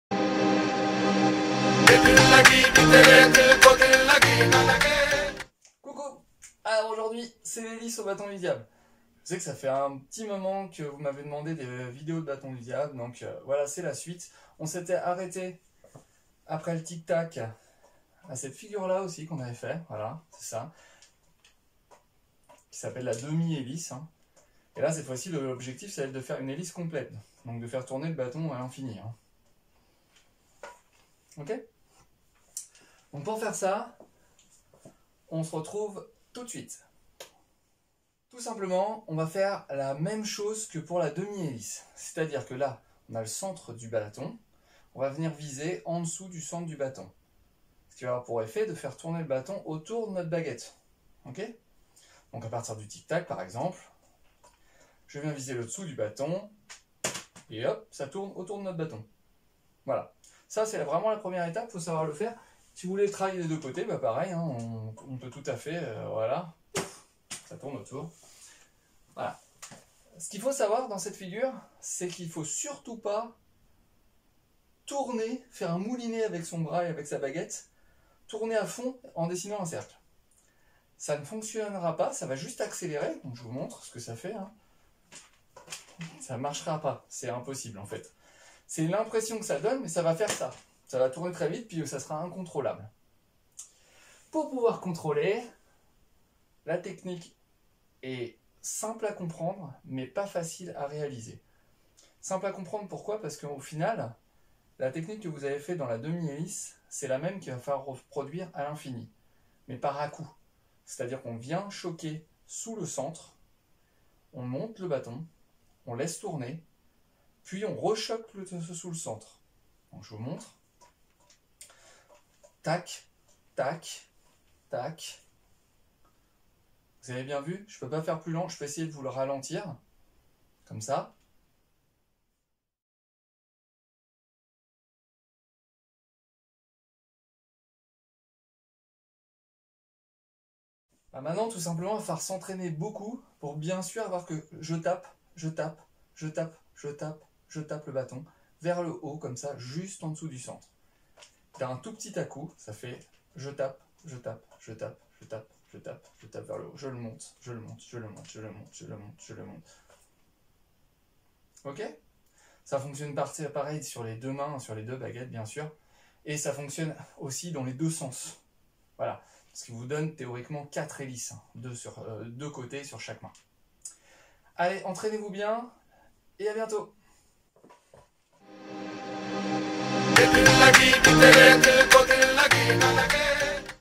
Tuto Bâton du Diable : l'hélice
Bon il y a quelques prérequis pour cette figure d'un niveau un peu plus complexe que les précédentes, il faudra donc bien maitriser au préalable deux mouvement important du bâton du diable qui sont  :

J'ai fait des vidéos pédagogiques su la chaîne pour chacune d'entre elle.